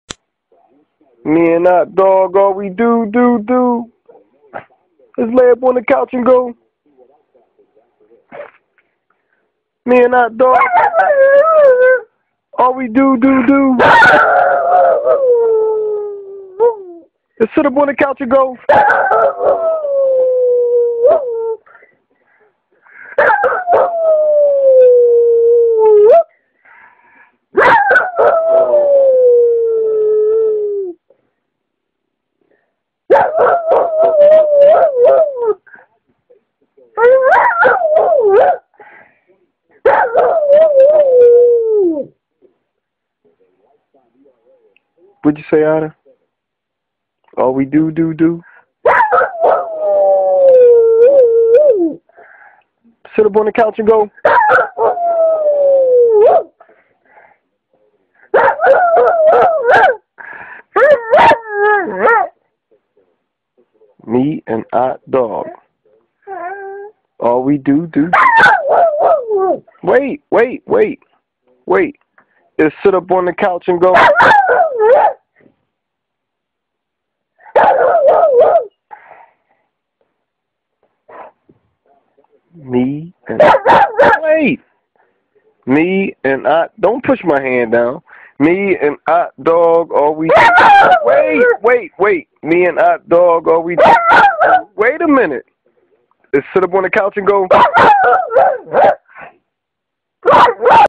Otter sings his hit song
"All me and Ott Dog are going to do, is sit up on the couch and go ru ru ru."